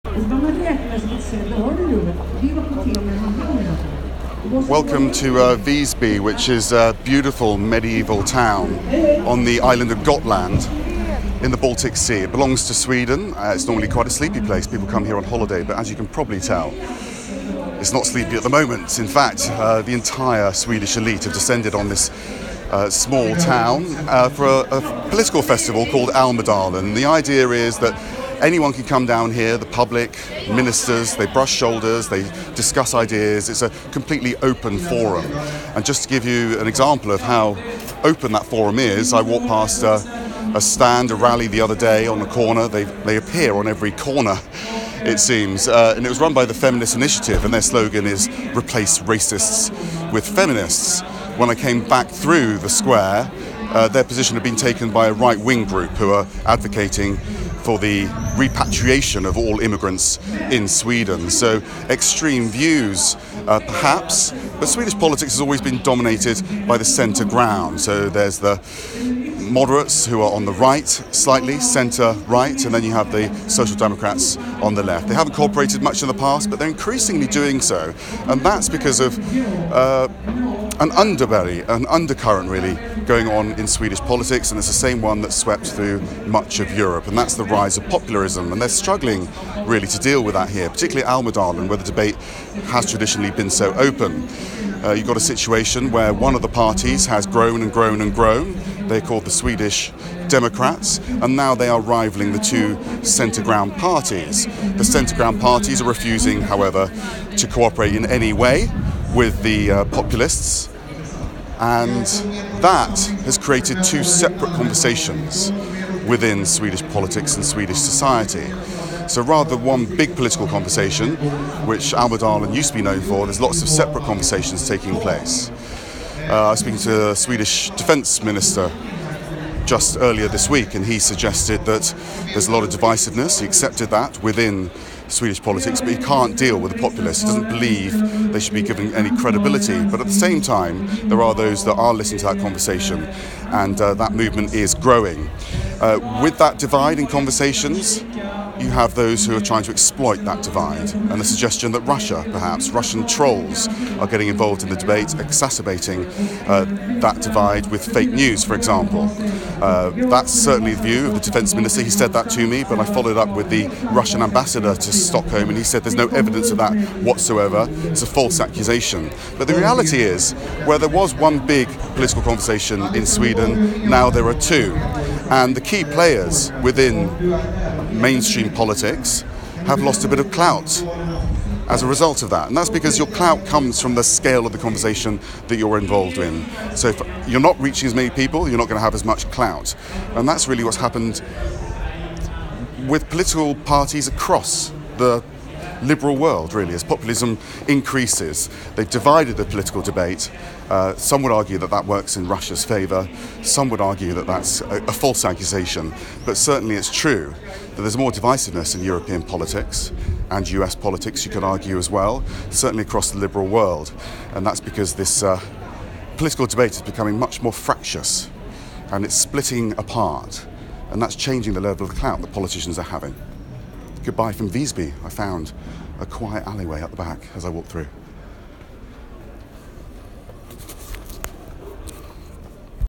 Populism, propaganda and political clout.
Max Foster travels to an idyllic Swedish island where the far-right are gaining political clout at the expense of the centre-ground.

I am in the medieval town of Visby on the island of...

It's all about clout.